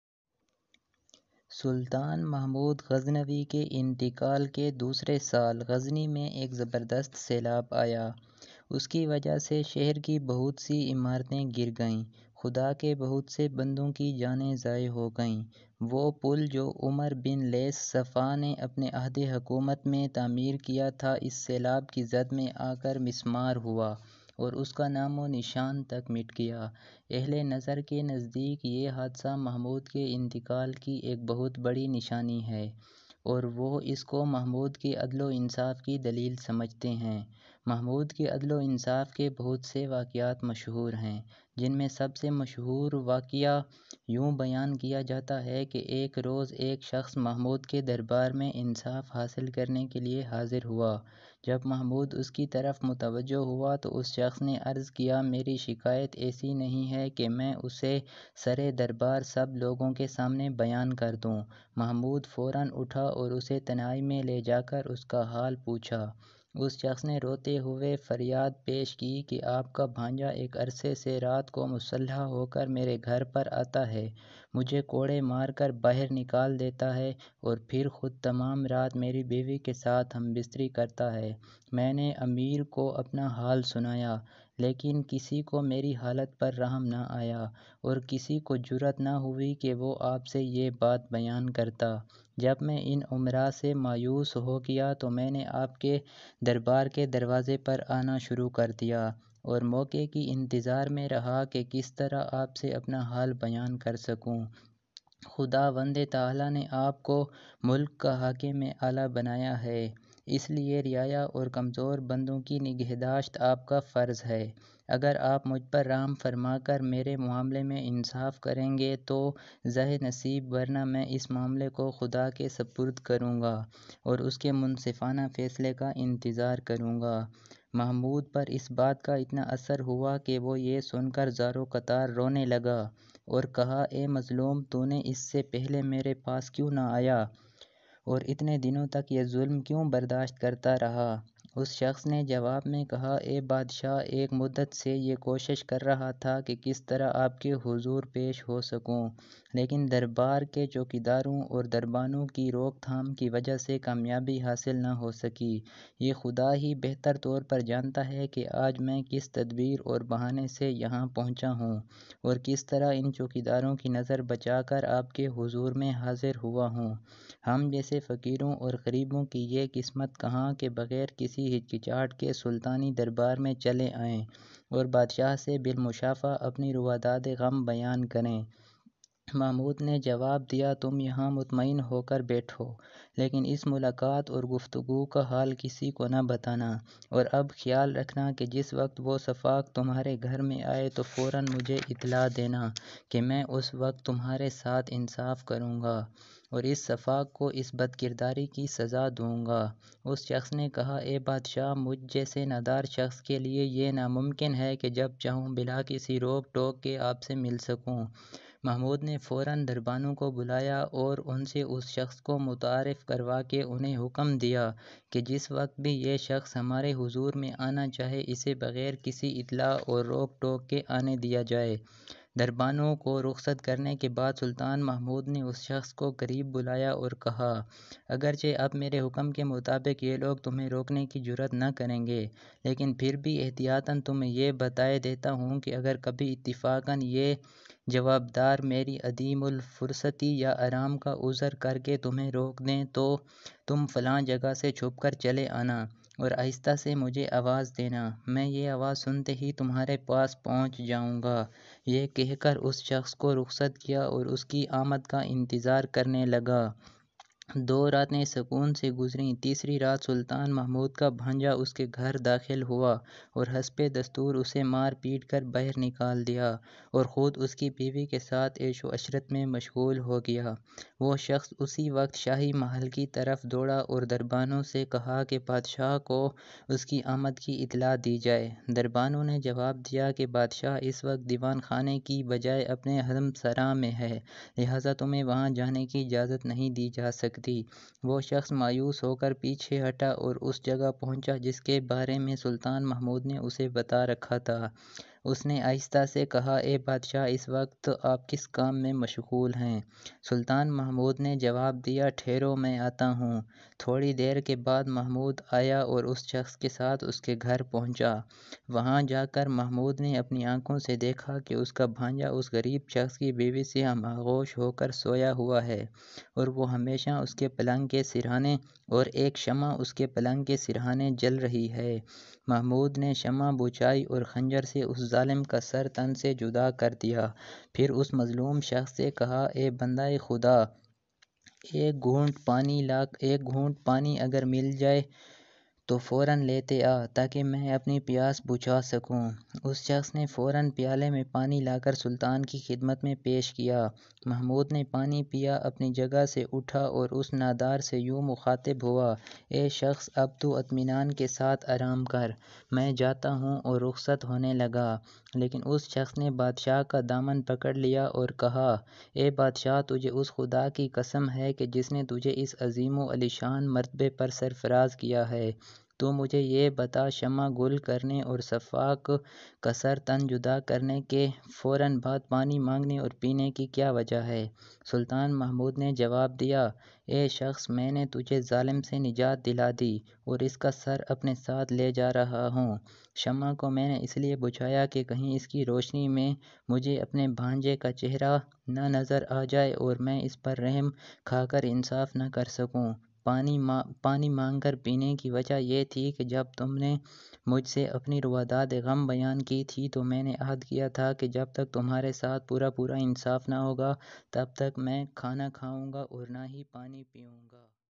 Sultan Mhemood Ghaznawi ke insaf ka yadgar waqia
In this video, there is a historical story of the king, Sultan Mhemood Ghaznavi.